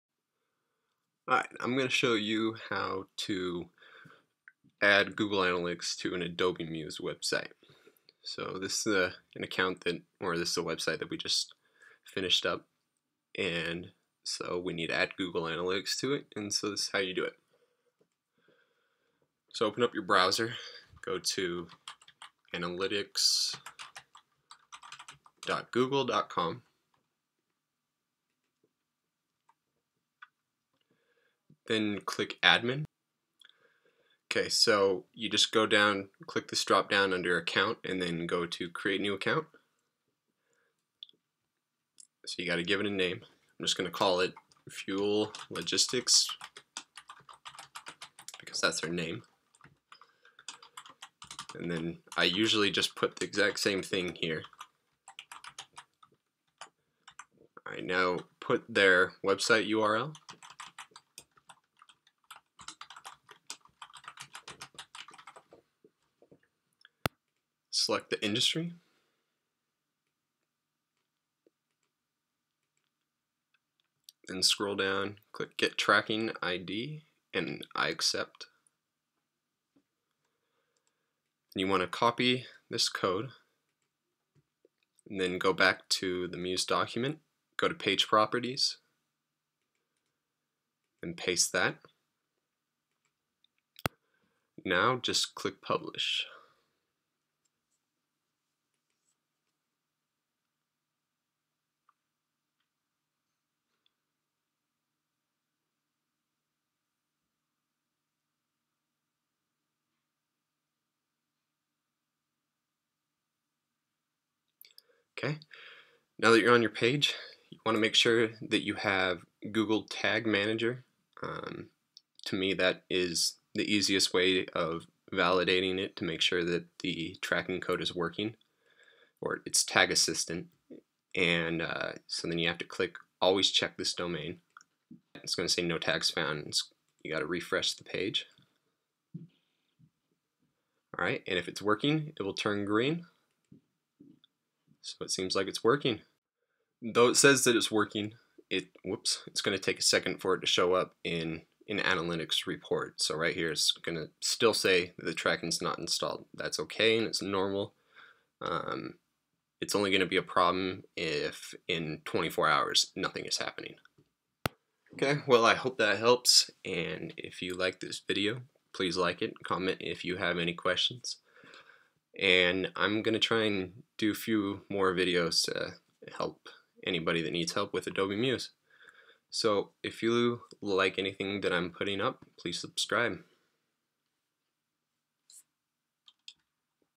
How To Setup/Install Google Analytics In Adobe Muse CC
I wanted to make this video because when I was first designing in Adobe Muse, I could not figure out how to get Google Analytics set up. It didn't help that I was trying to install the updated version of their analytics called "Universal Analytics". I say that it didn't help only because I couldn't find any kind of tutorial or anyone confirming that it could be done.

So this post is to let you know, yes, Google Analytics can be installed into Adobe Muse. Here's how.

If you prefer to read the text version of this post, go